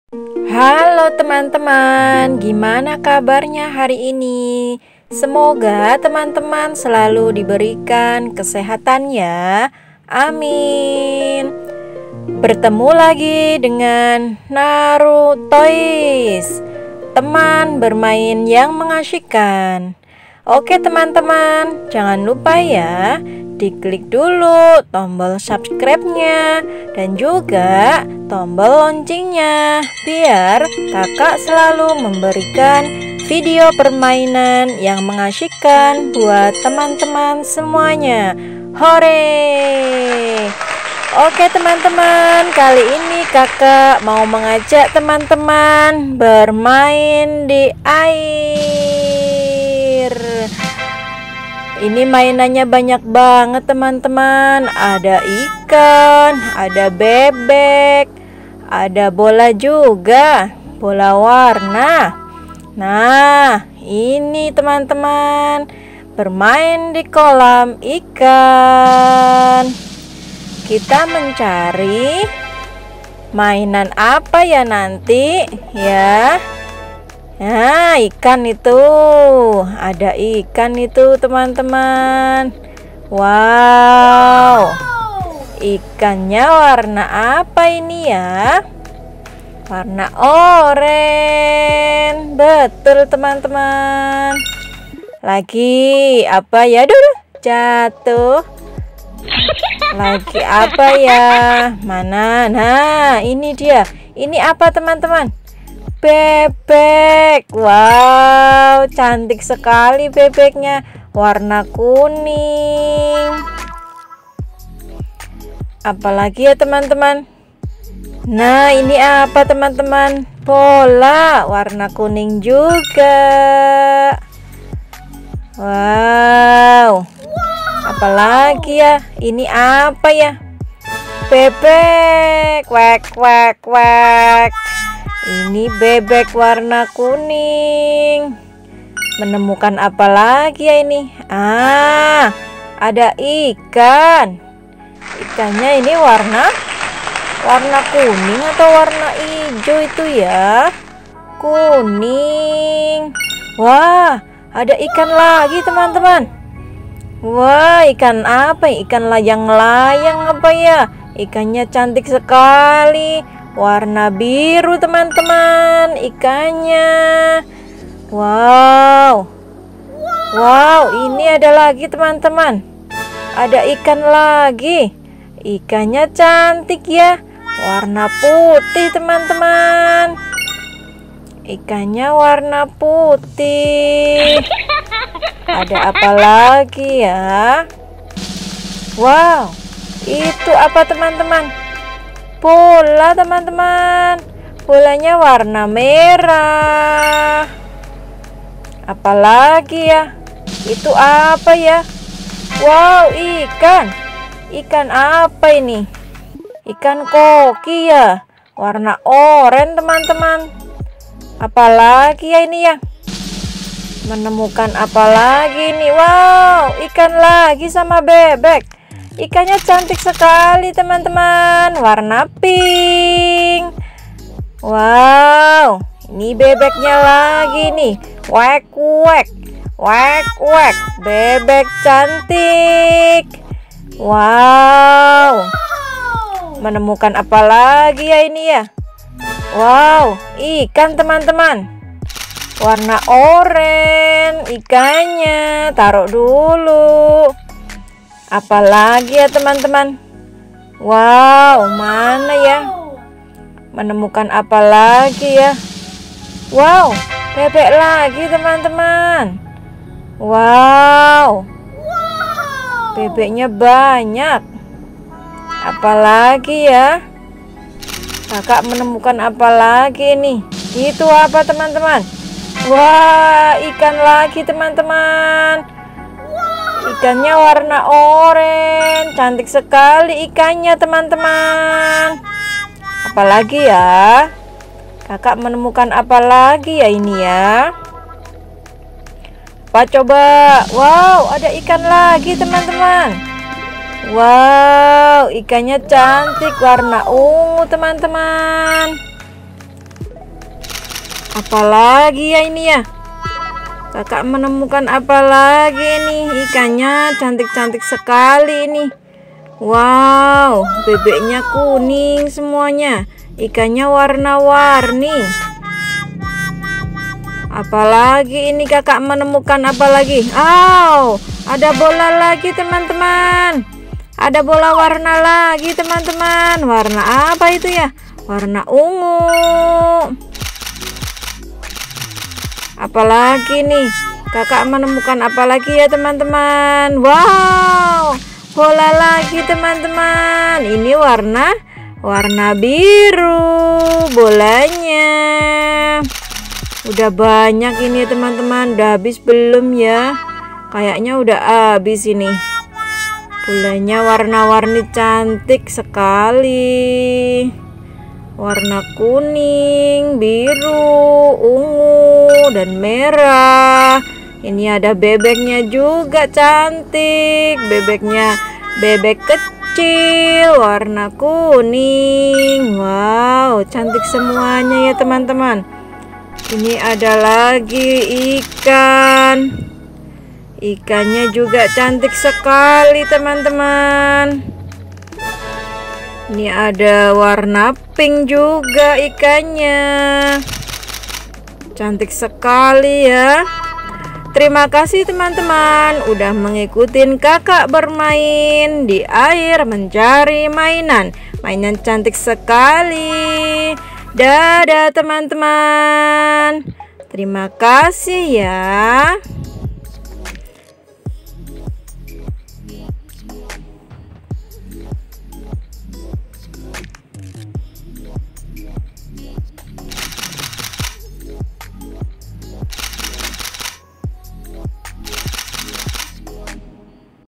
Play in the fish pond, look for fish, find fish, ducks, colorful balls, chef fish, ornamental fish
Halo..
Teman-teman apa kabar hari ini, semoga selalu diberikan kesehatan yaaa Aamiin..
Kakak mau mengajak teman-teman bermain dikolam ikan, mencari ikan, menemukan ikan, bebek, dan bola warna warni 😍 yuuuk ikutin kakak yaaaa teman-teman.. ‼️

Pemusik: Jeff Kaale